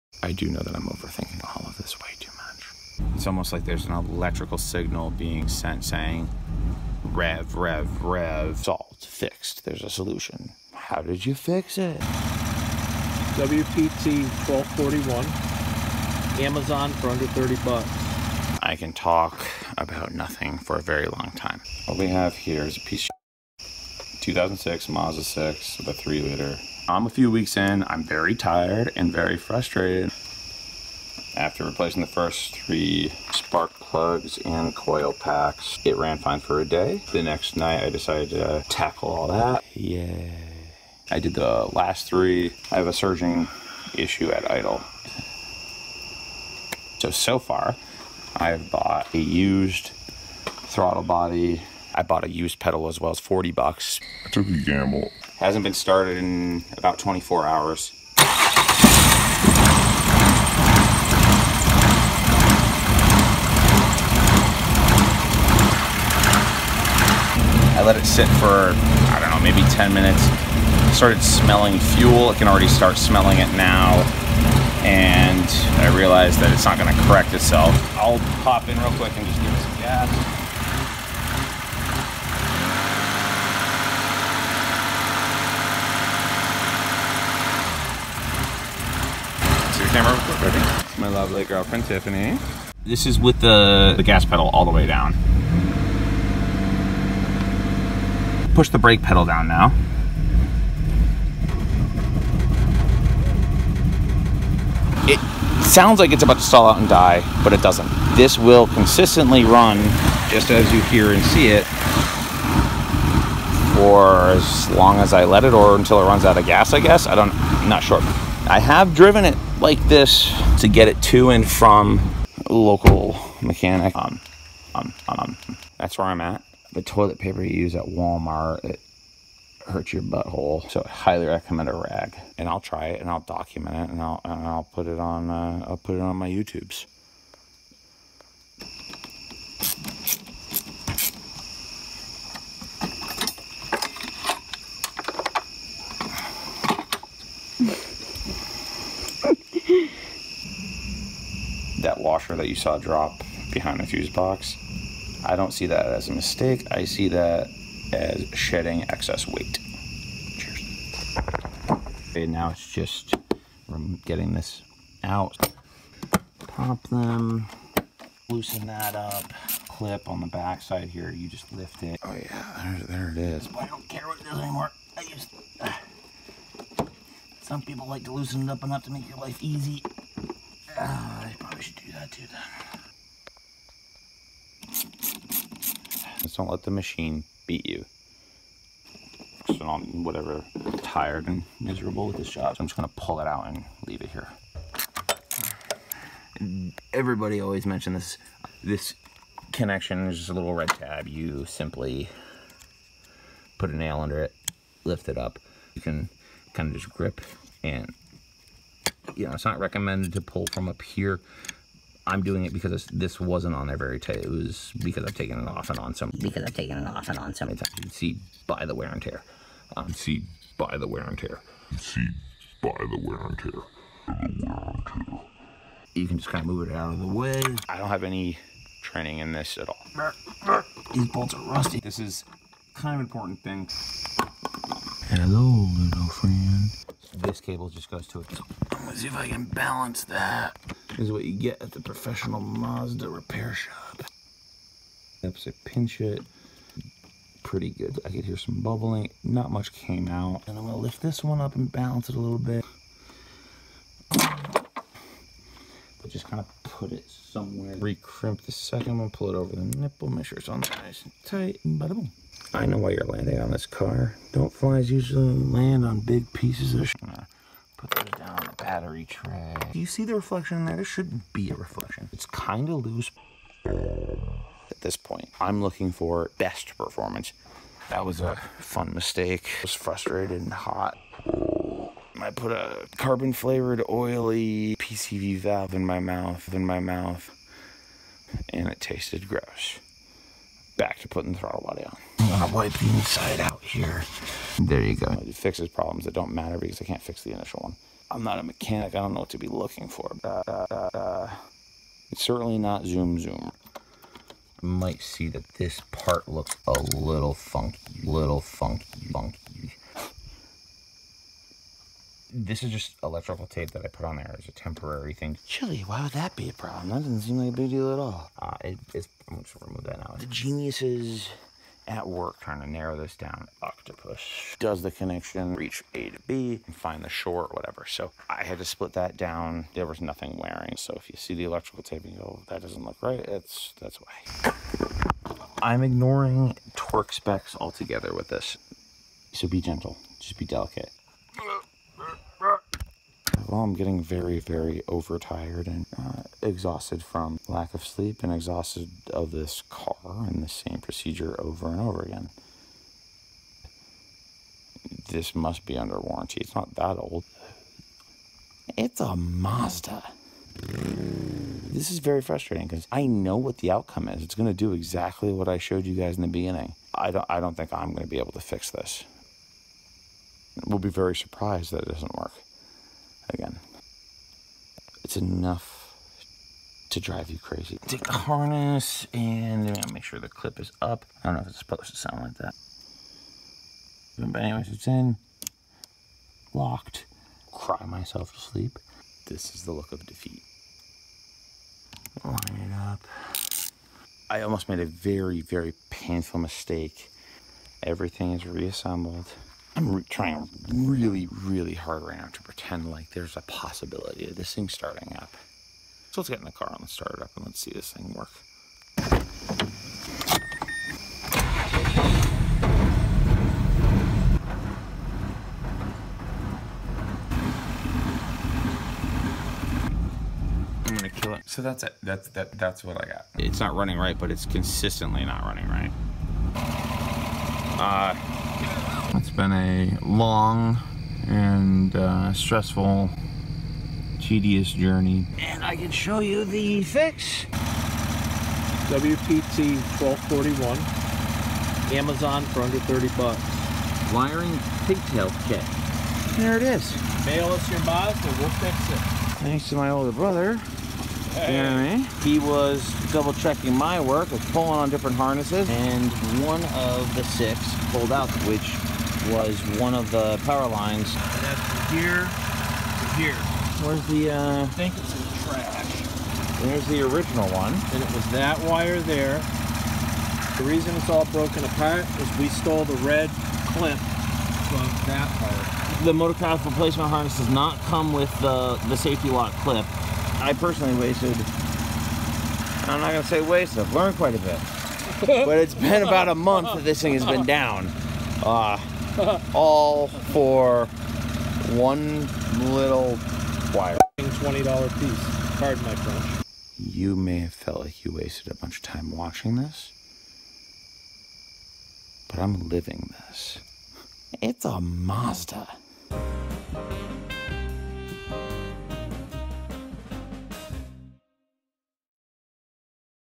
2006 Mazda 6 Busted Pigtail Harness to Throttle Body = Major Idle Headache
After trying to do some diy at home repairs on my car, which I know nothing about, I caused more problems than there were to begin with. I stumbled my way through spark plug changes, gasket replacements and electrical nightmares that lead me to replacing my throttle body and accelerator pedal. Here's a not so fun look into my driveway disaster project. After a couple weeks of messing around I decided to start documenting the rest which I have to edit down into more focused "episodes". Enjoy my pain and hey, maybe this will help someone else out in a similar boat. I had a tough time finding anything similar in my searches while trying to figure this puzzle out. I'm in no way a mechanic and trust me. it shows.